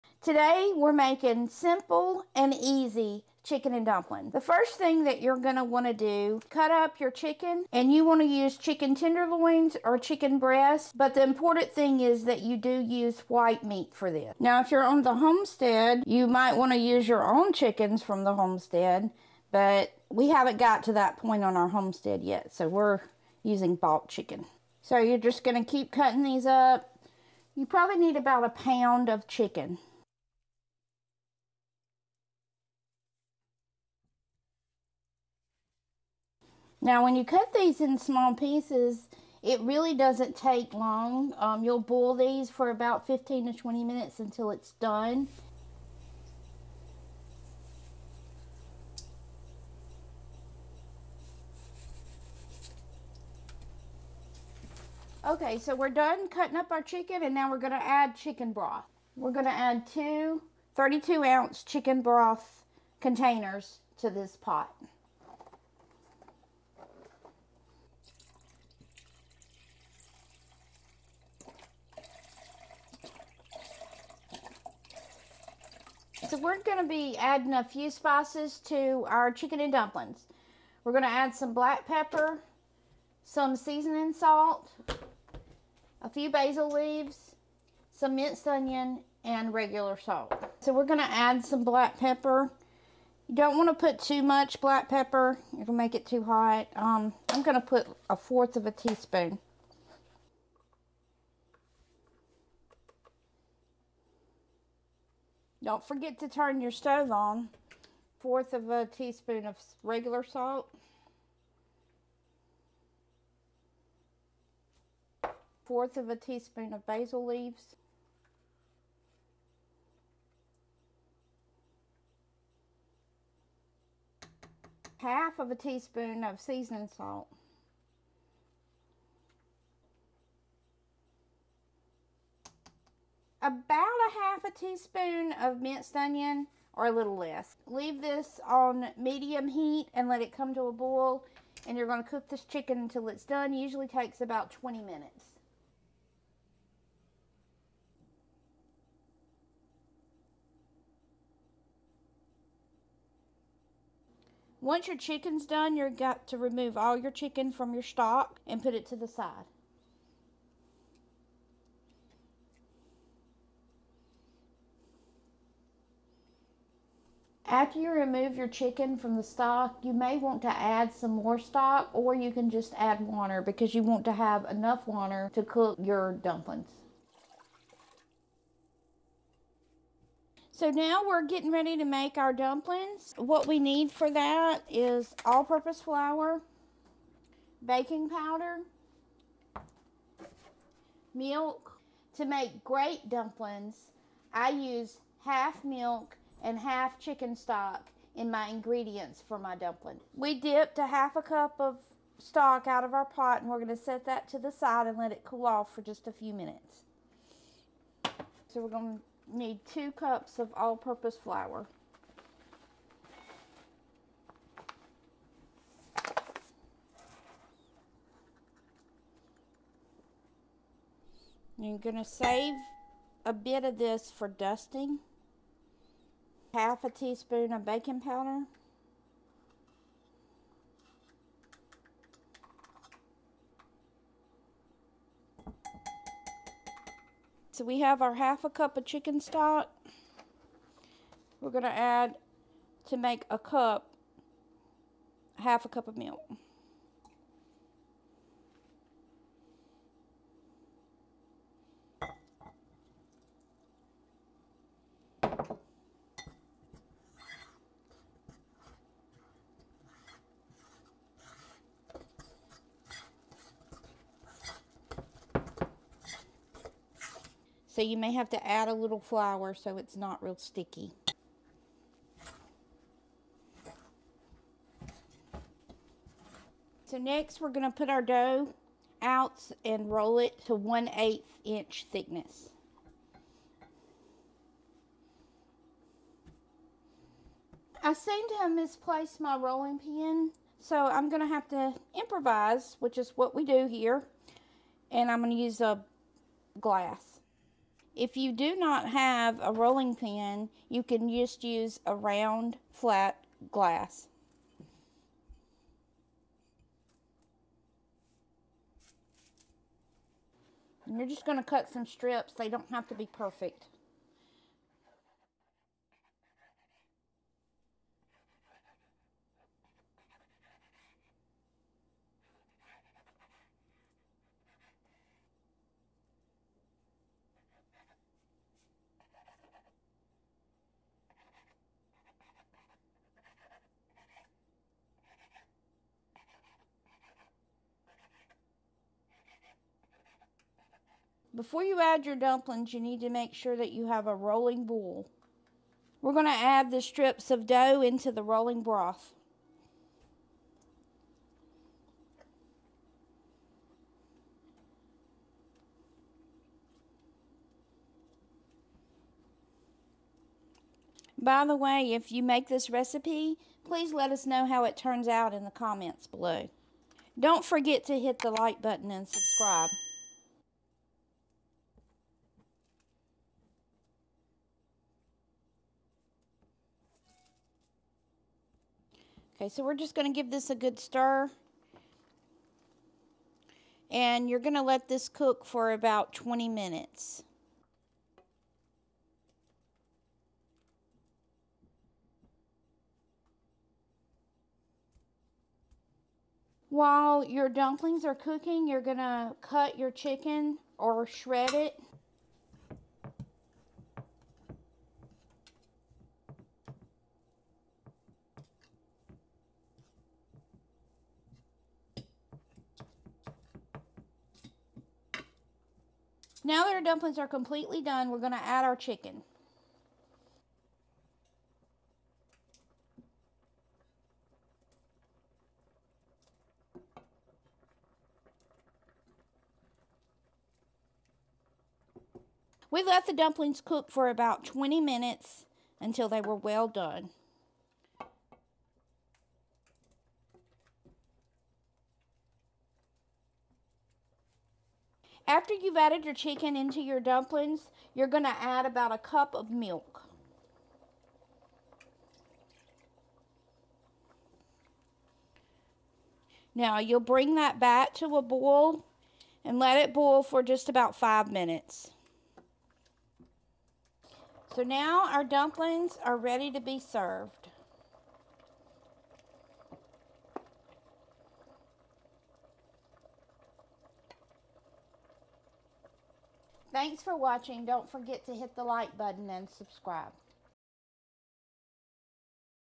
How To Make Homemade Chicken and Dumplings!!! Southern Style
Learn how to make the BEST homemade chicken n dumplins recipe you will every have!!

Ingredients:
1to 2lbs chicken breast
8 cups of chicken broth
1/4 teaspoon back pepper
1/4 teaspoon salt
1/4 teaspoon chopped parsley
1/2 teaspoon seasoned salt
1/2 teaspoon minced onion
1 cup milk (added the last 5 minutes)

For Dumplings
1/2 cup milk
1/2 cup chicken broth
2 cups all purpose flour
1/2 teaspoon baking powder